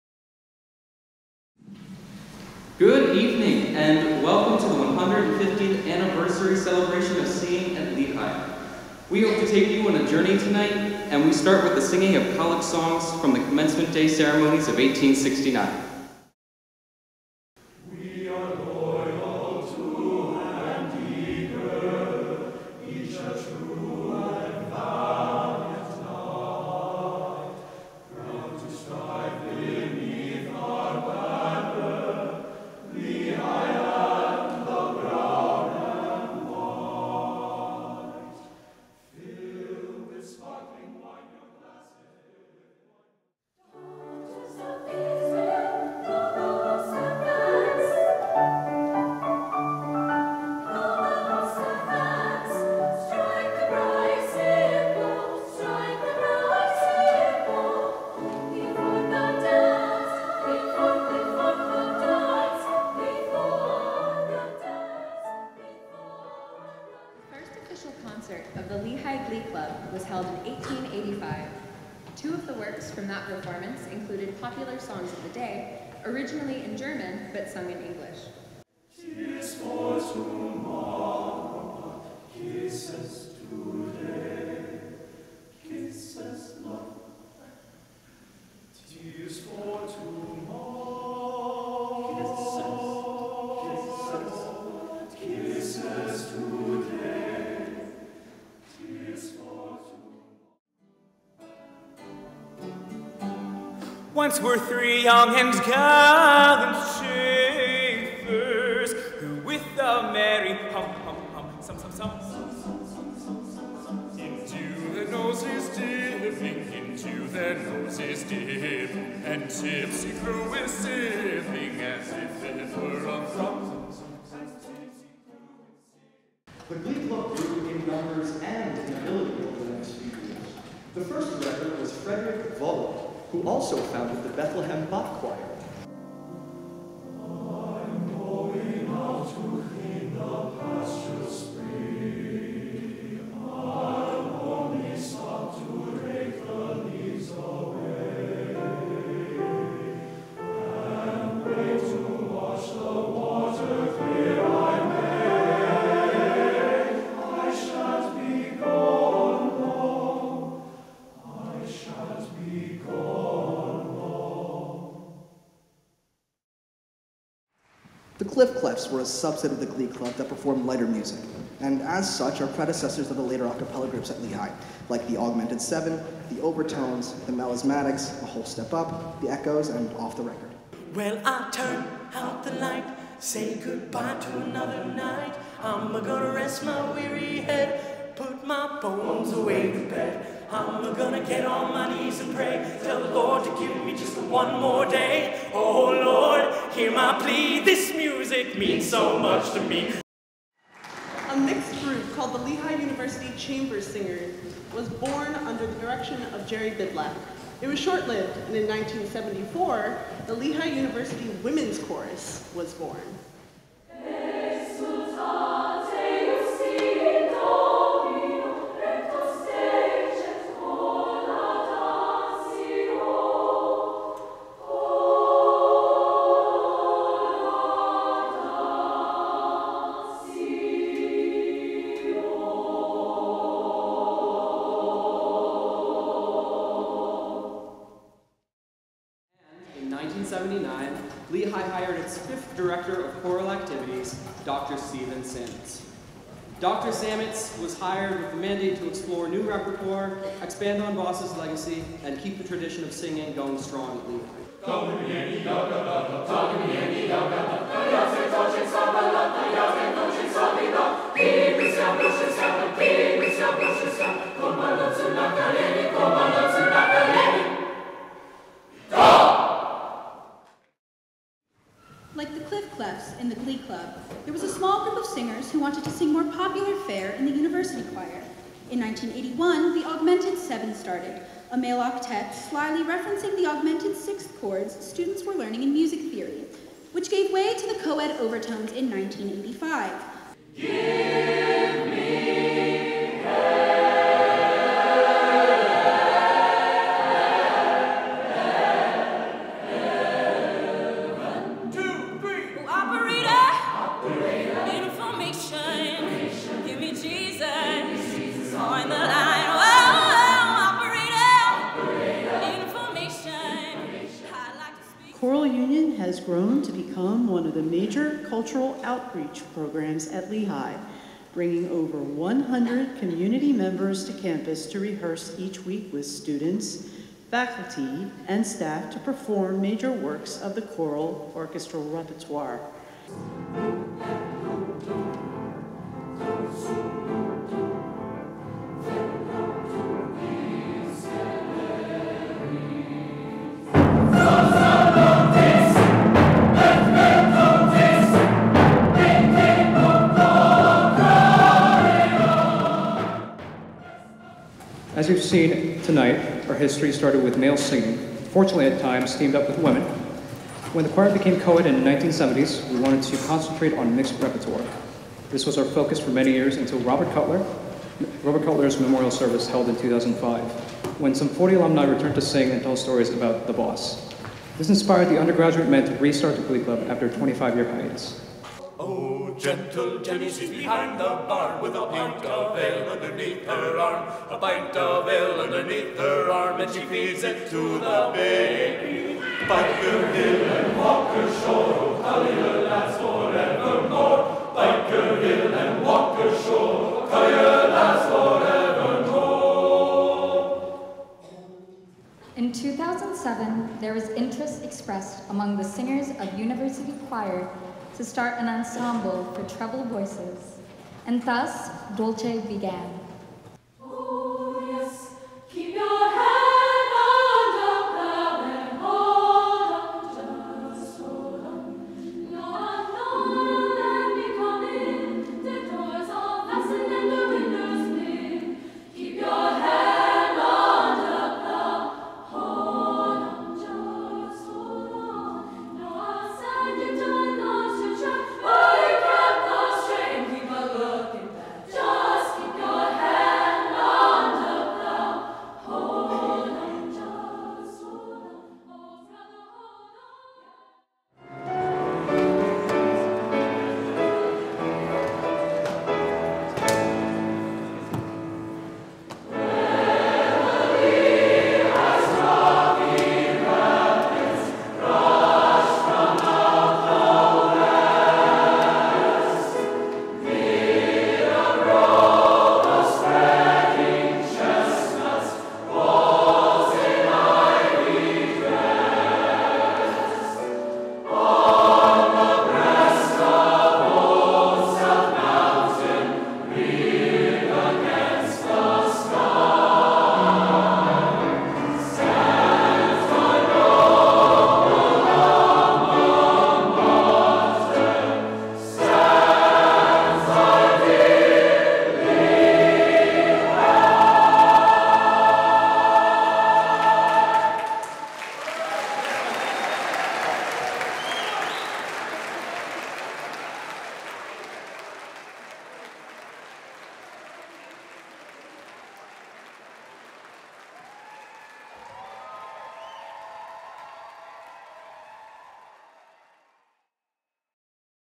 Shine On! A Celebration of 150 years of singing at Lehigh University
Lehigh University Music Department
Director(s)
Steven Sametz ; Sun Min Lee ; Paula Zerkle ; Steven Mathiesen

Performer(s)
Moravian College Choir women ; Lehigh Percussion Ensemble ; Lehigh University Glee Club ; Cliff Clefs ; Lehigh University Women's Chorus ; Lehigh University Choir ; The Overtones ; Lehigh University Choral Union ; Dolce

Theatre Location
Baker Hall, Zoellner Arts Center

Performance Dates
Friday, March 29, 2019 ; Saturday, March 30, 2019

Steven Lichak
Digital Media Studios
Lehigh University

Composer(s)
Sigismond Ritter von Neukomm ; Oswald Lorenz ; Friedrich HieronymousTruhn ; Randall Thompson ; Josquin des Prez ; Carter-Nevada ; Lodovico Grossi da Viadana ; Leonard Lechner ; Steven Sametz ; Henry Purcell ; Carl Orff ; David Dickau ; Jing Ling-Tam ; Kevin Memley ; Carlos Chavez ; Casey Rule ; Reena Esmail

Document Type
Program

Publication Date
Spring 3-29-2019

Recommended Citation
Lehigh University Music Department, "Shine On! A Celebration of 150 years of singing at Lehigh University" (2019). Performance Programs. 607.

Download
PLUMX METRICS
INCLUDED IN
Music Performance Commons